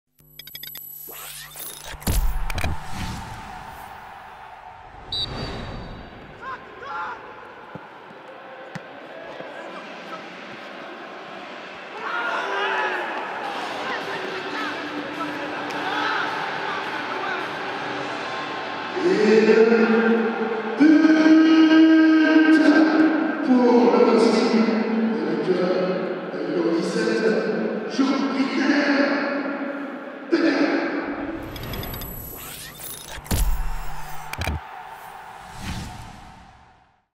Le but de Jeanricner Bellegarde face au RC Lens
Vu du bord terrain, le but de Jeanricner Bellegarde lors de la réception du RC Lens (1-2) pour le compte de la 30ème journée de Ligue 1 Uber Eats 2020/2021.